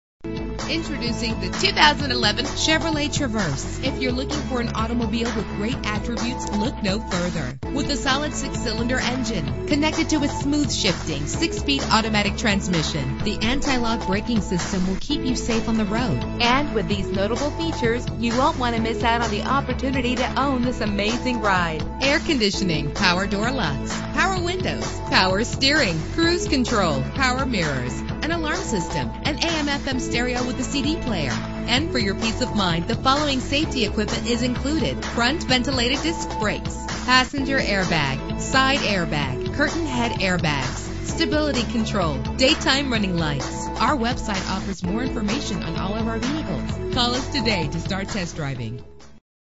2011 Chevrolet Traverse - White Bear Lake MN
Year: 2011
Make: Chevrolet
Model: Traverse
Trim: 1LT
Engine: 3.6 liter 6 cylinder 24 valve
Transmission: 6-Speed Automatic
Color: White Diamond Tricoat
Mileage: 165
Address:
1801 E. County Road F